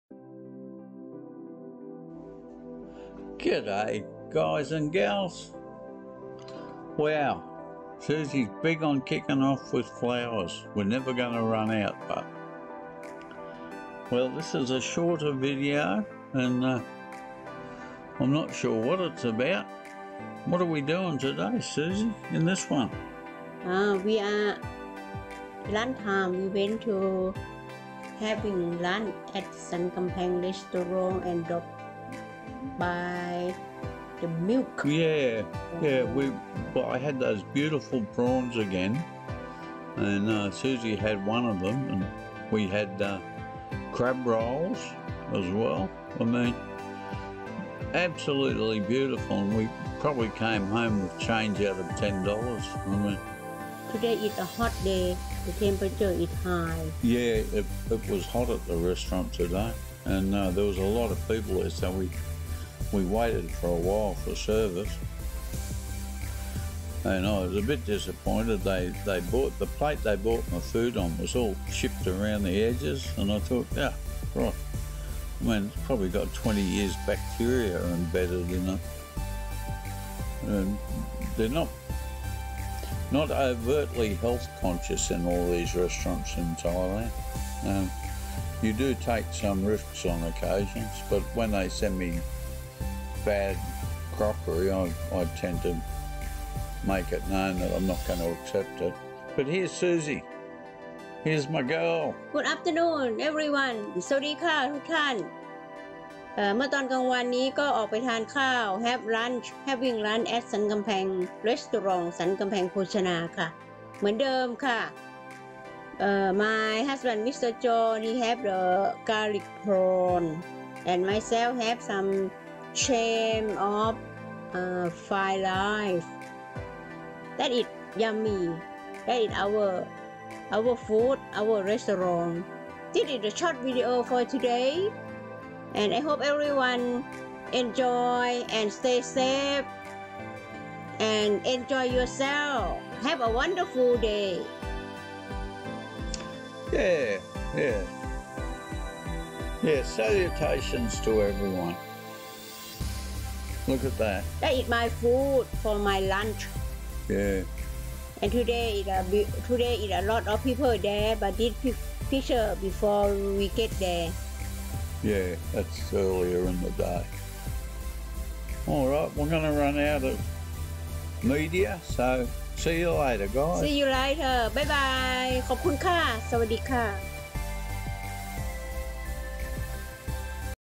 great sea food 1000 ks from the oceon
Thai - Aussies lifestyle for John & Susi in Chiang Mai , in On- Tai Sankamphang    Have a wonderful  week day everyone  วันนี้  เล่าเหตุการณ์เมื่อตอนบ่าย ออกจากบ้านไปทานอาหารกลางวันที่ร้านสันกำแพงโภชนา คือ กิจกรรมของลุงจอห์นและ สุภาลักษณ์ ผู้สูงอายุ ให้ทุกท่าน ชม เพื่อ สนุก และ เพลิดเพลิน แบ่งปันความสุขค่ะ ขอให้ทุกท่านปลอดภัยและโชคดี มีสุขภาพแข็งแรงค่ะ